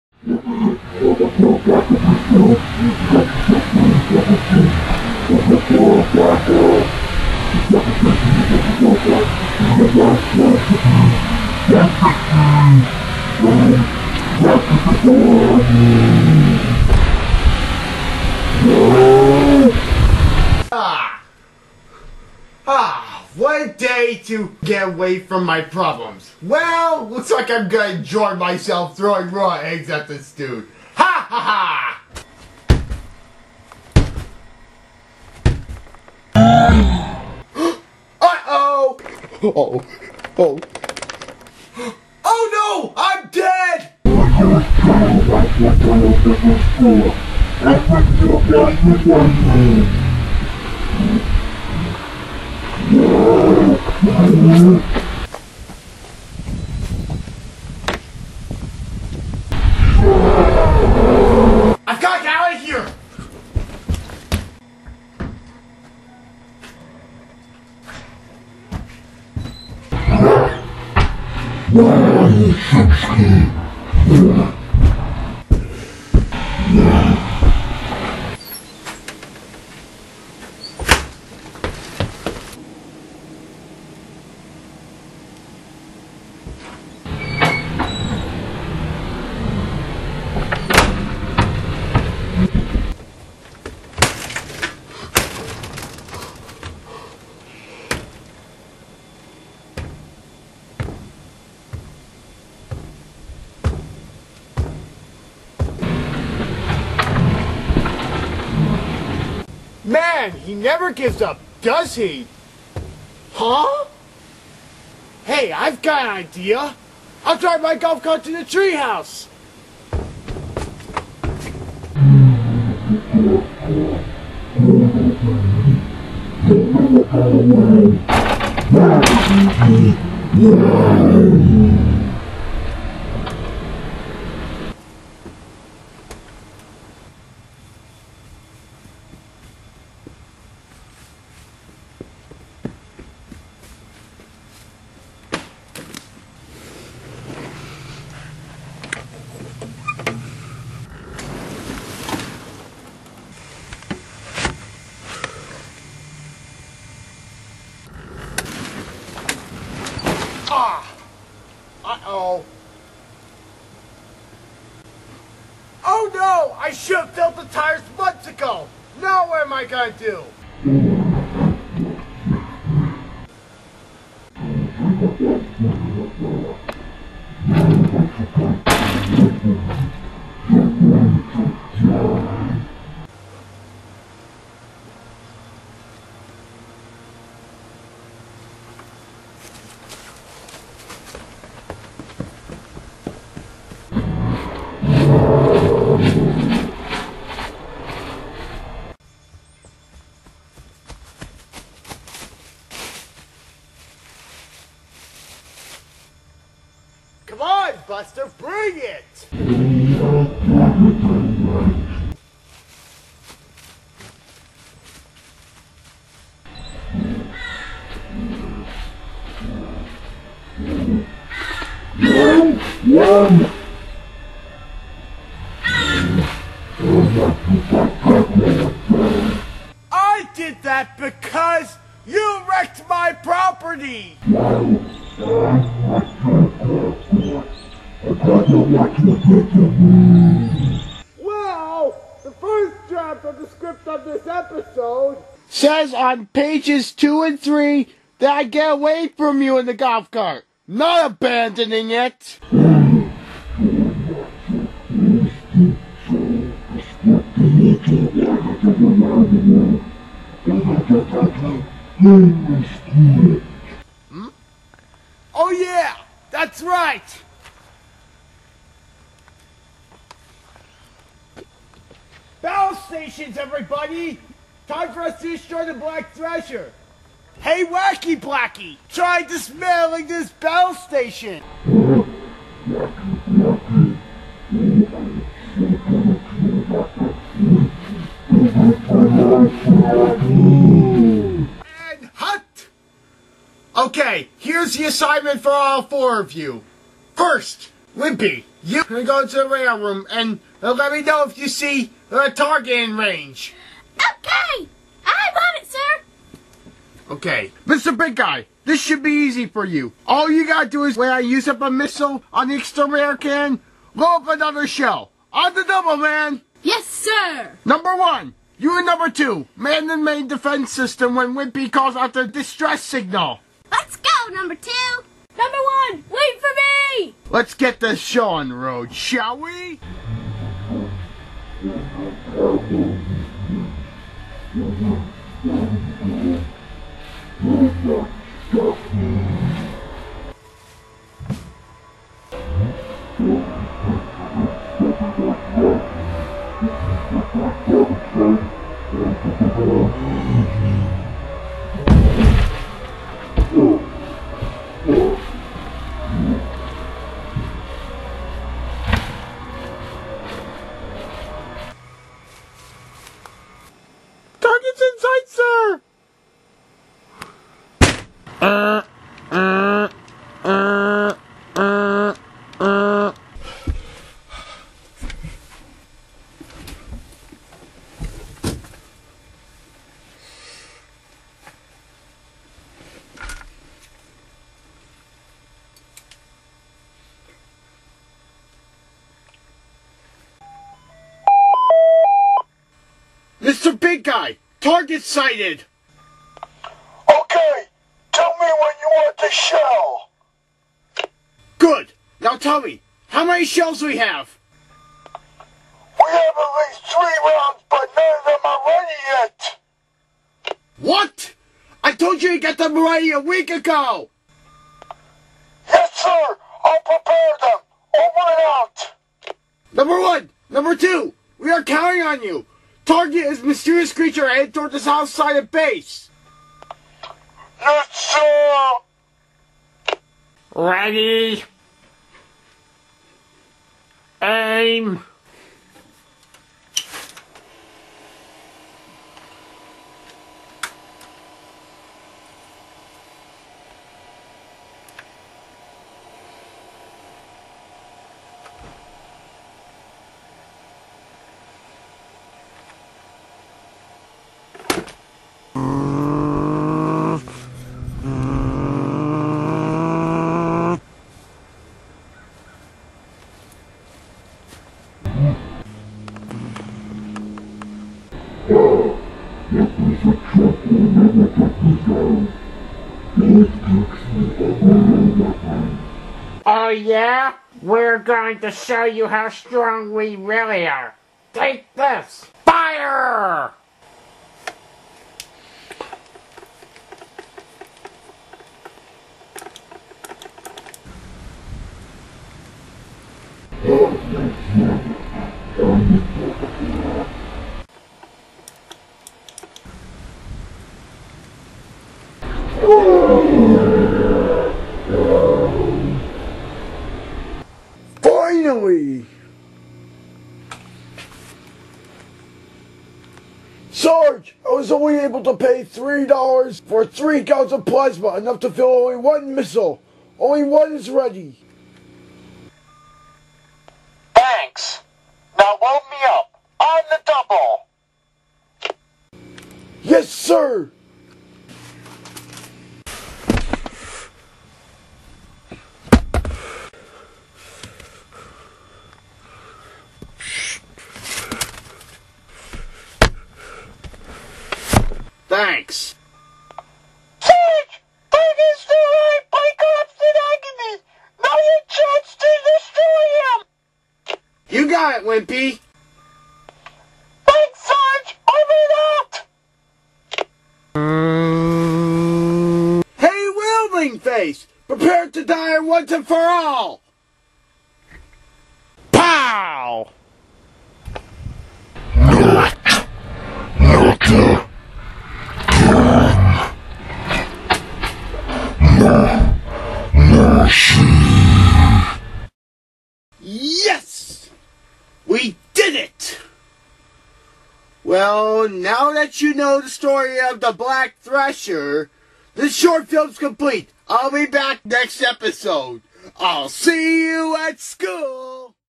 Brand new voice for the evil Black Thrasher (Part 3/3)
Here's the conclusion to the original 2007 Black Thrasher films (complete with Black Thrasher's new voice).
the adventure of the black stallion,
thrasher movie,
evil thrasher,